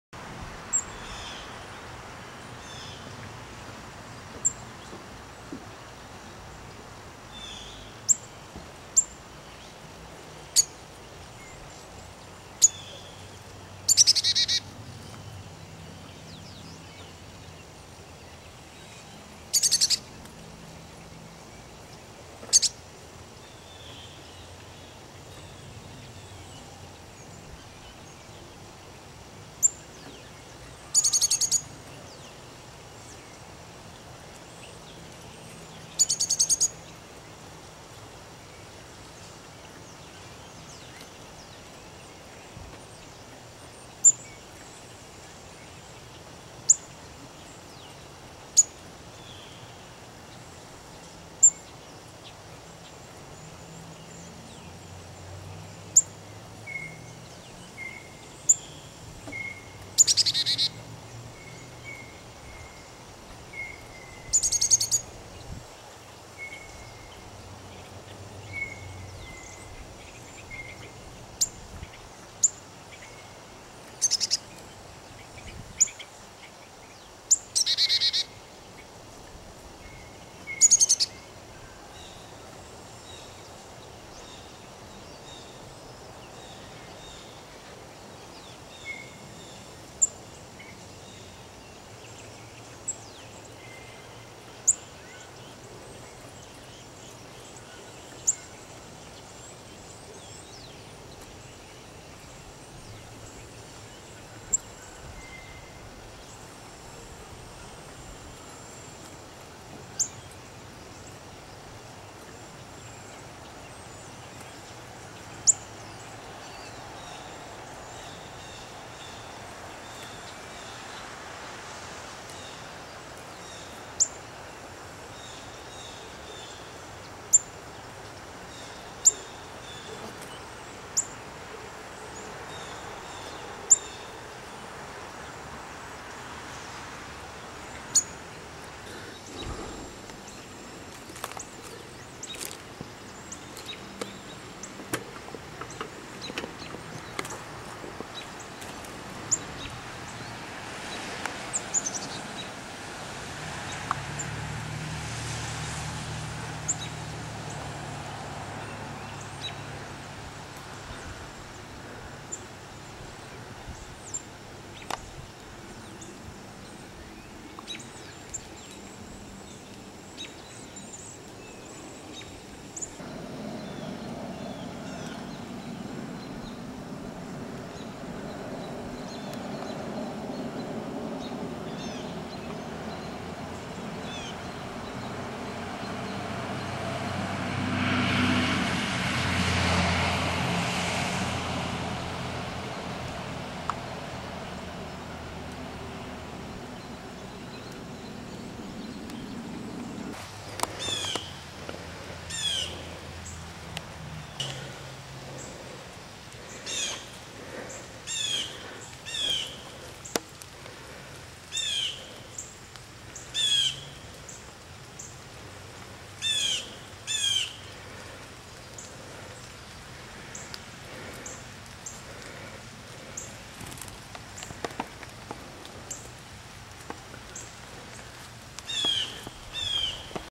Chickadee Release
This is an adult Carolina Chickadee that I found in my yard almost a week ago unable to fly.  I took it to a local wildlife rehabilitator and within a week it was ready for release.  It apparently hit a window and had an air sack in it's chest...common for birds that strike windows.  I picked it up from the rehabber and placed the cage on my deck for a bit so it could get used to it's surroundings.  I then opened the cage and let it fly away but it ended up towards the front fo my yard and close to the main road.  I was able to re-capture it and place it in the woods behind my home.